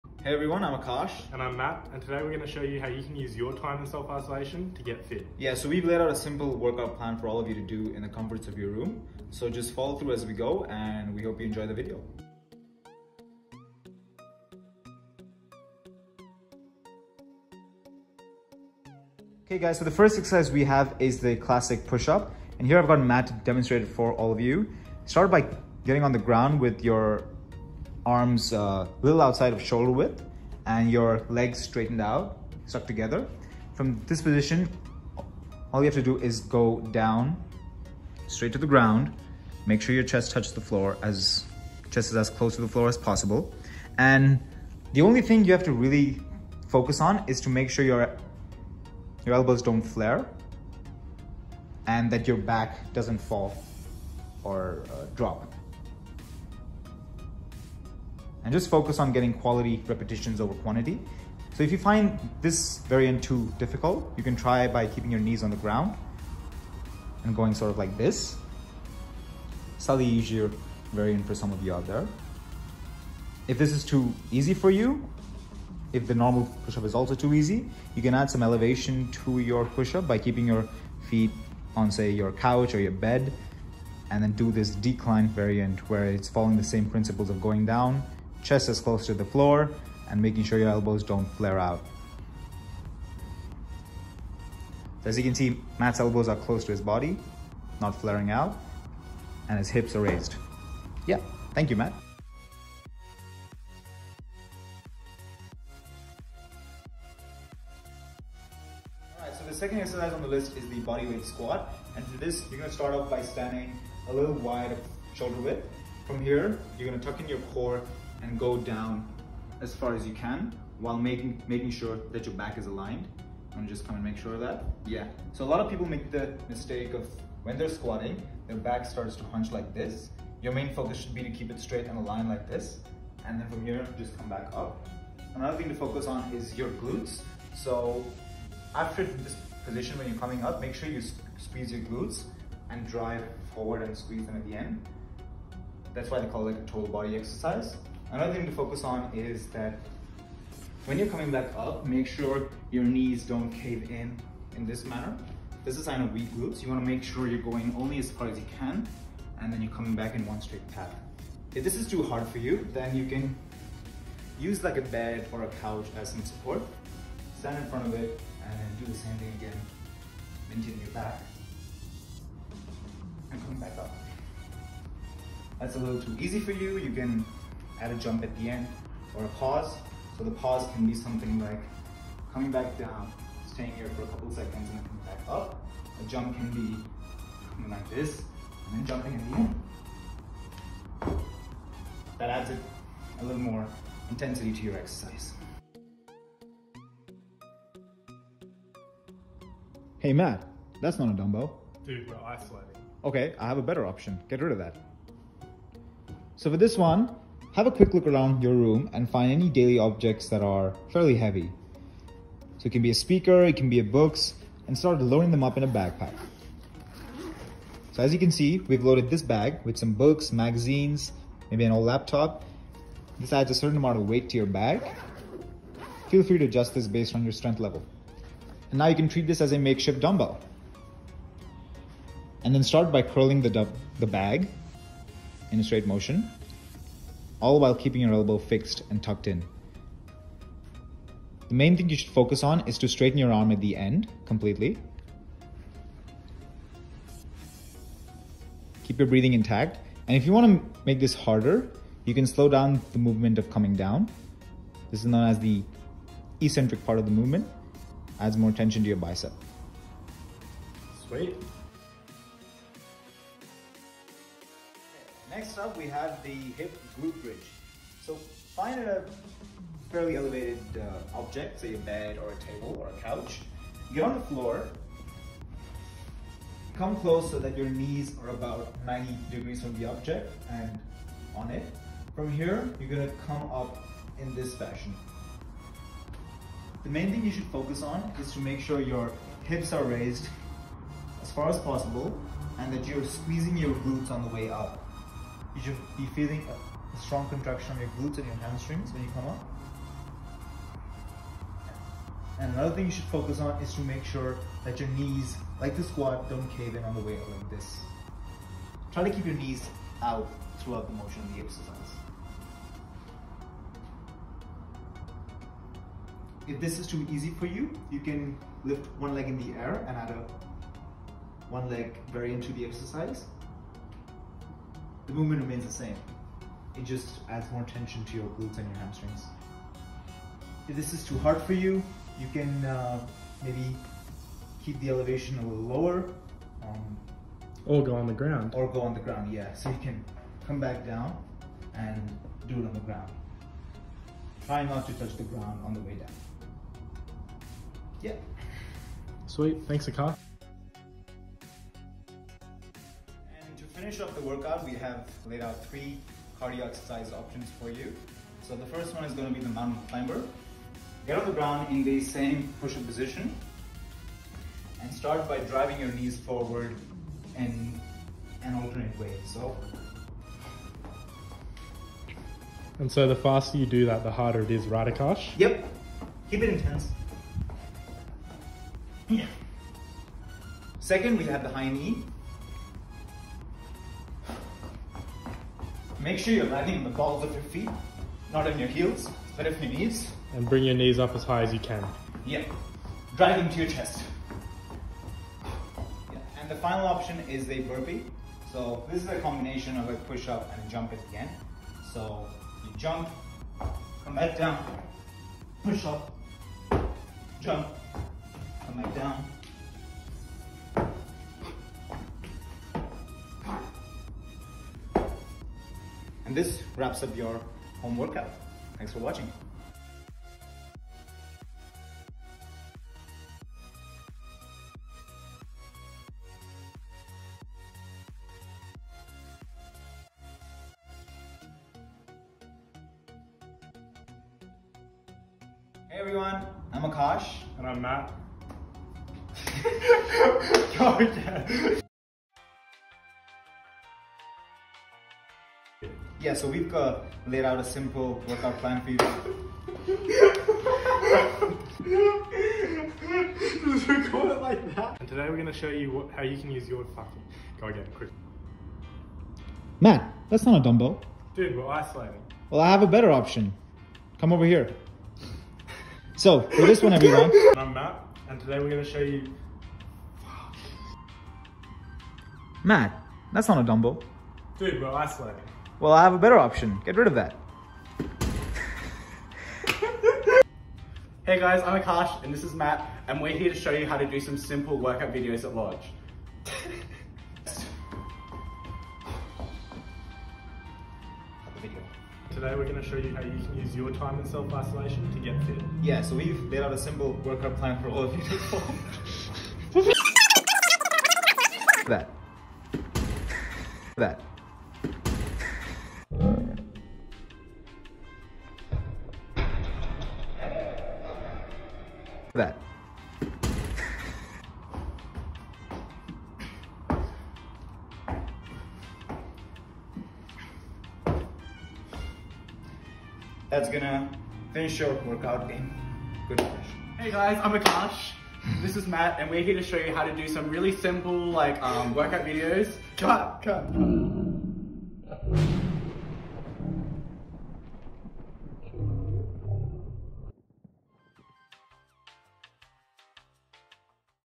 Home Workout
Join Mat and Akash from LK as they take you through a workout you can do in your own home!